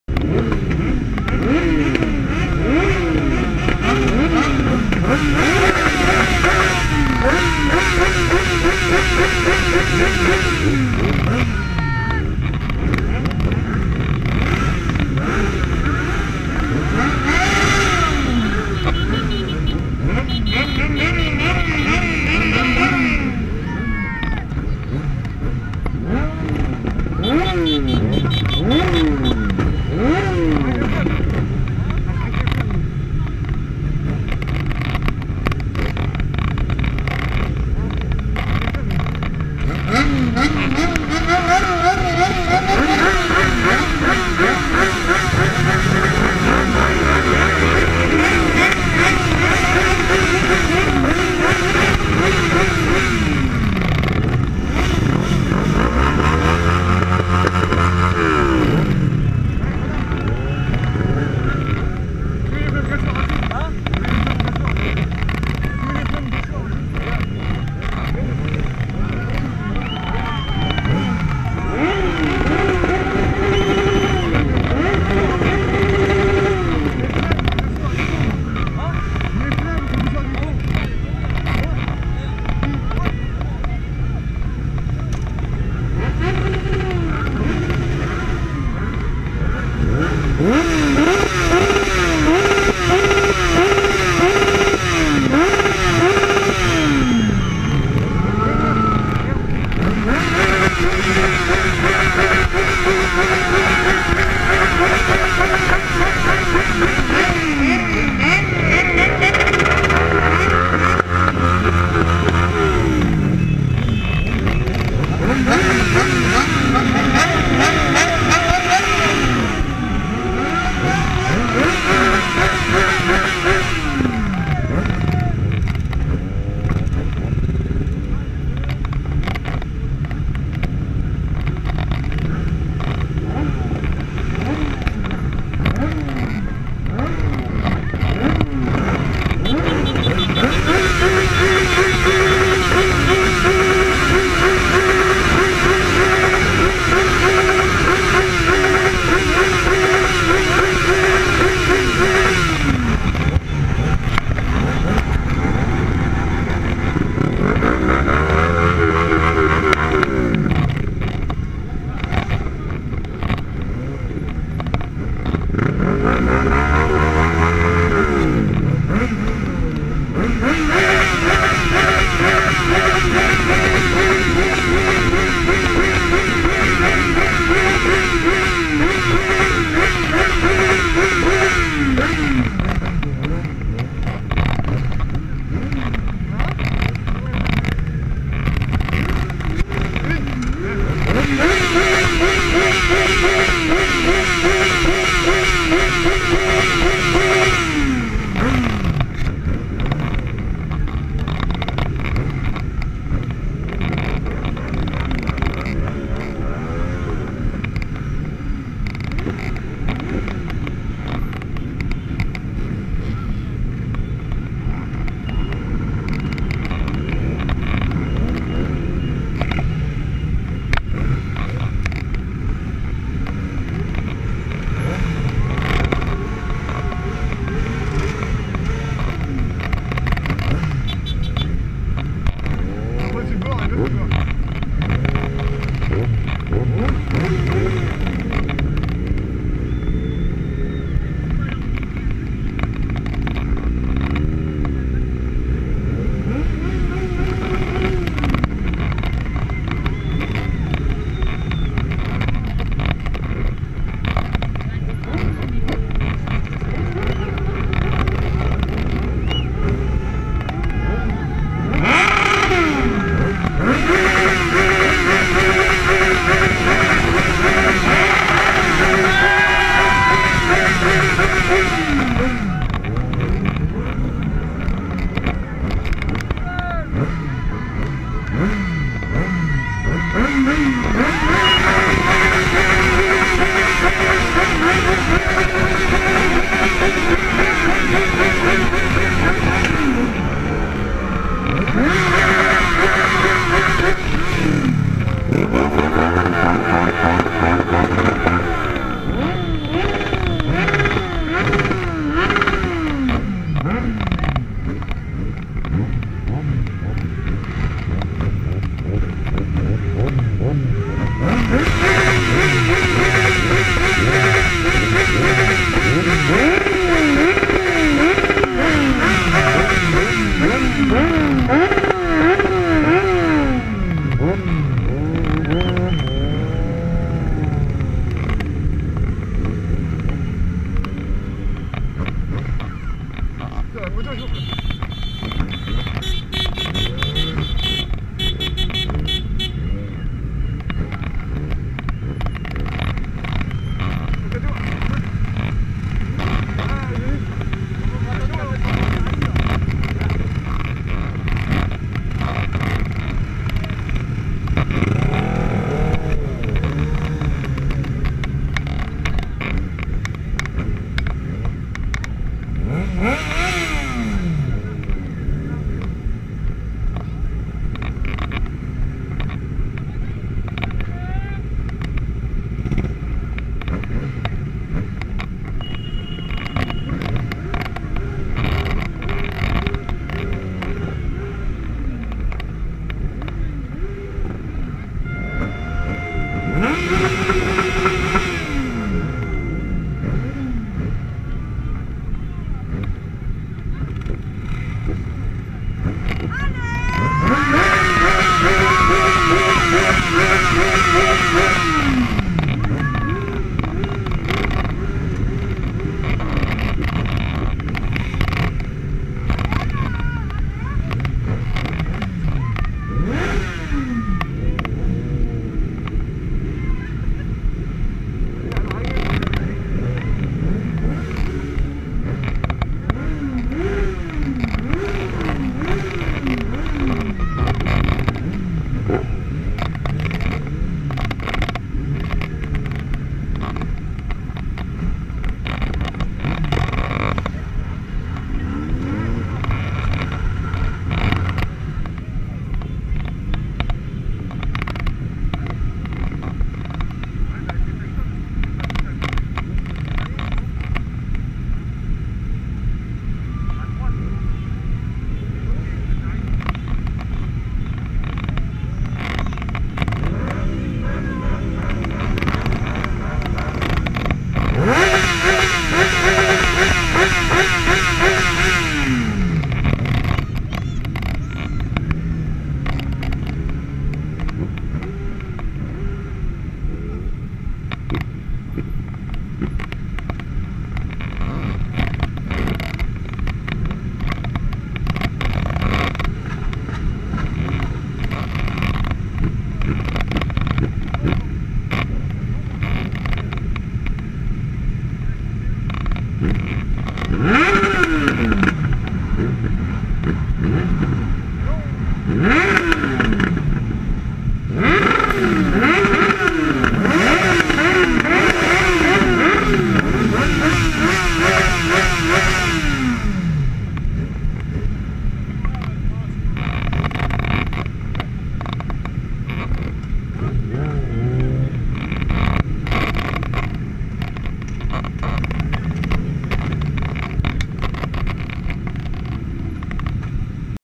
descente moto fécamp 2013, partie 2
vidéo de la descente moto de fécamp 2013 embarqué sur un XJ6.